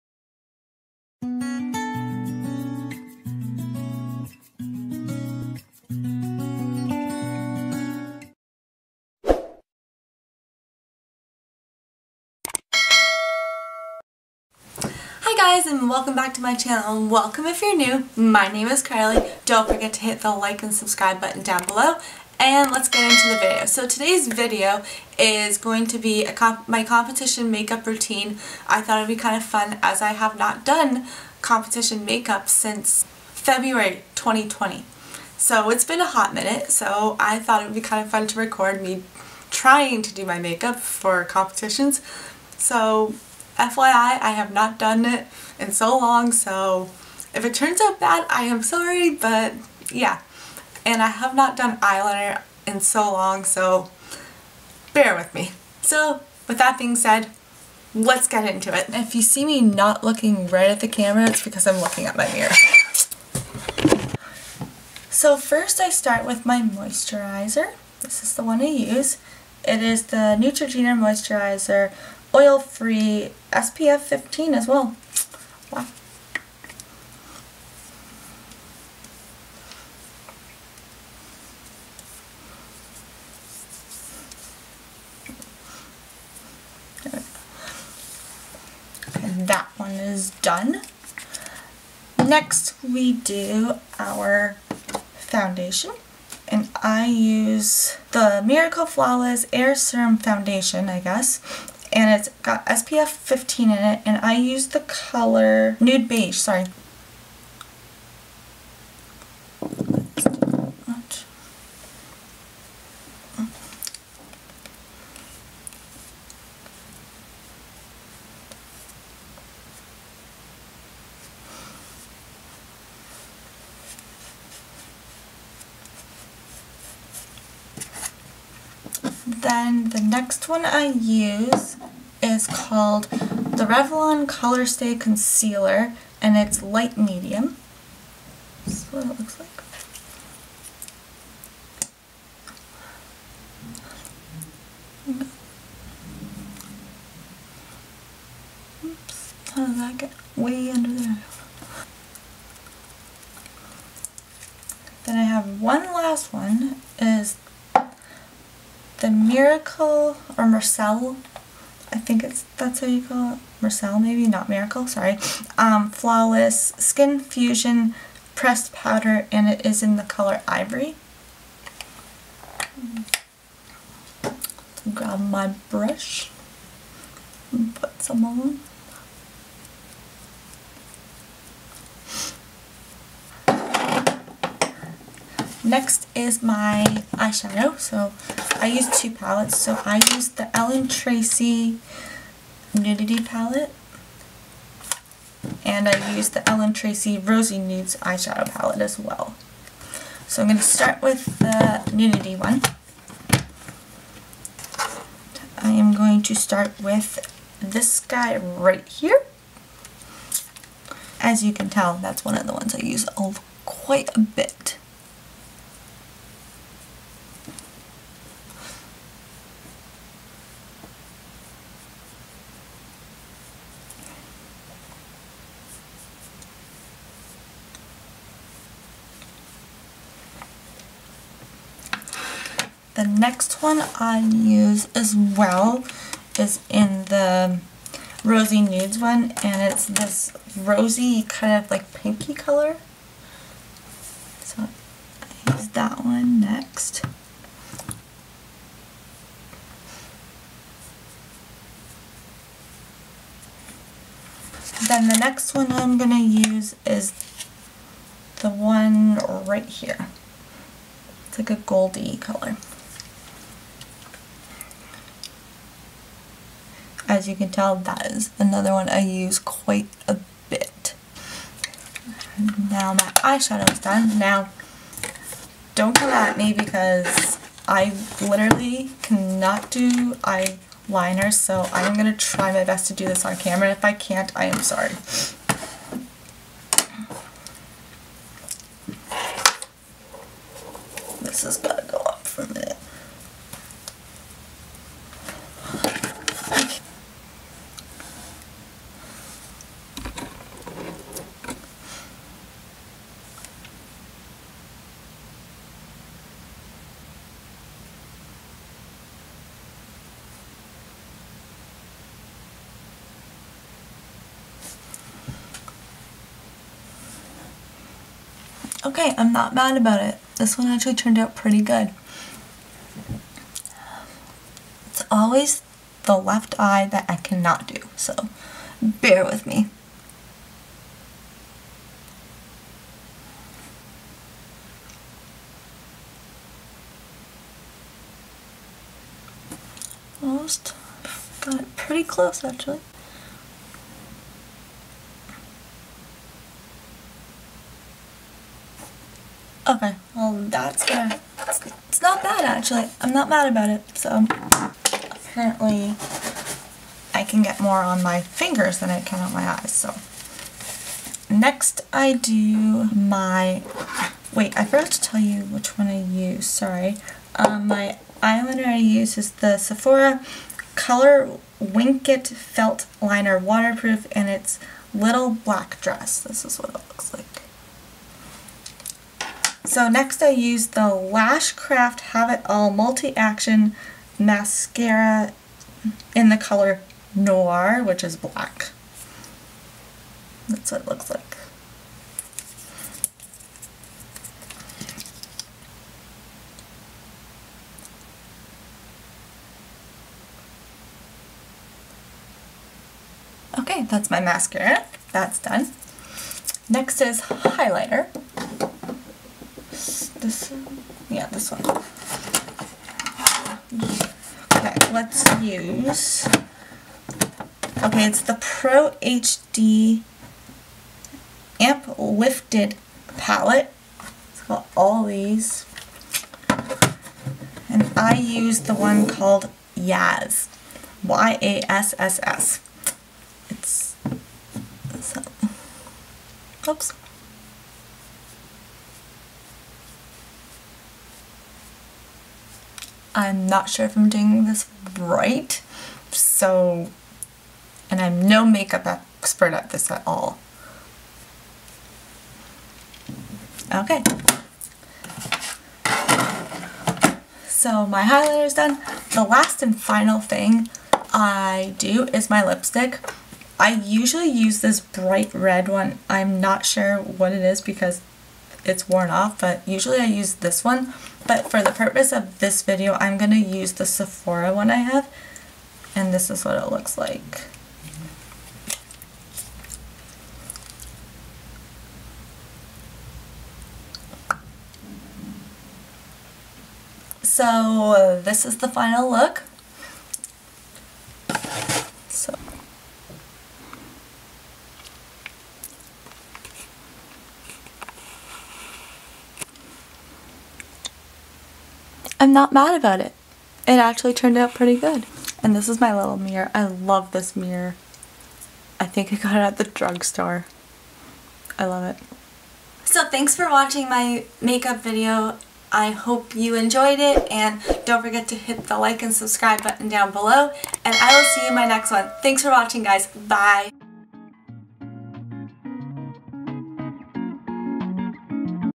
COMPETITION MAKEUP ROUTINE!!! I FIGURE SKATING EDITION!!
Here is my competition makeup routine for my figure skating competitions. I have not done my competition make up routine in over a year.